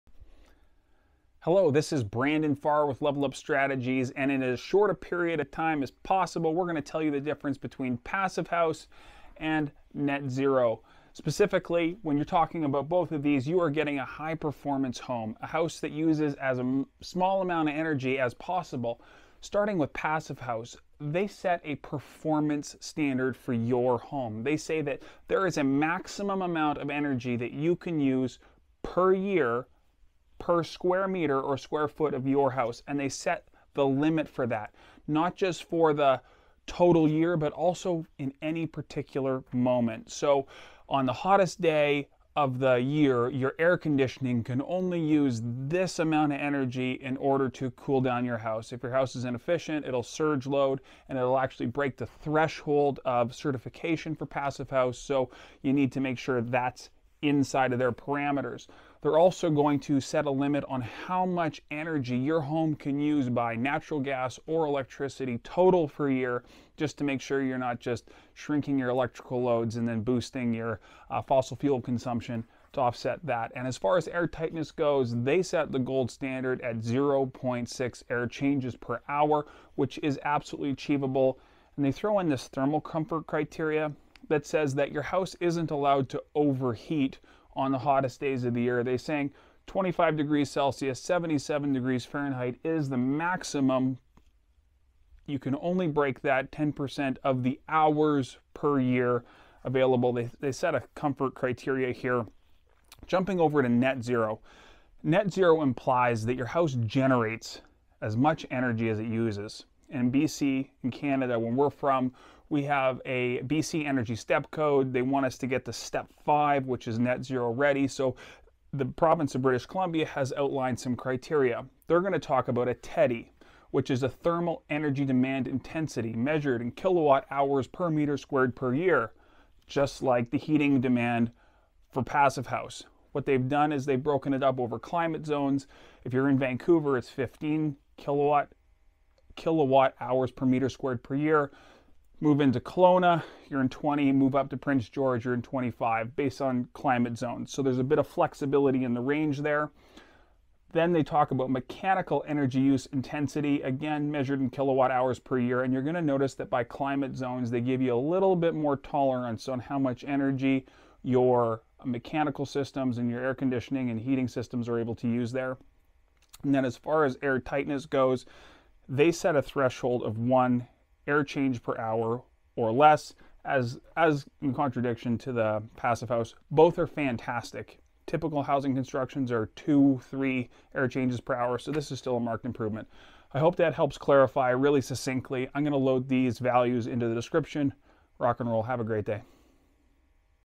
Passive House vs Step 5 Net Zero Ready House in 200 seconds or less
Here it is - in as tight a package as possible.   Hopefully this clarifies some things for people looking to contrast the different ways of getting to a high-performance house.

Passive House
Heating/Cooling Demand = 15 kWh/m2*yr
Heating Demand = 10 W/m2
Primary Energy Demand = 120 kWh/yr
Air Tightness = 0.6 ACH or less
Thermal Comfort = 25*C/77*F   +/- 10% hours/yr

Step 5 Net Zero Ready
Thermal Energy Demand Intensity (TEDI) kWh/m2*yr.   15/20/25
Mechanical Energy Use Intensity (MEUI) kWh/m2*yr     25/30/40
Air Tightness = 1.0  ACH or less

0:00 Introduction
0:20 Passive House Described
1:43 Net Zero Ready Described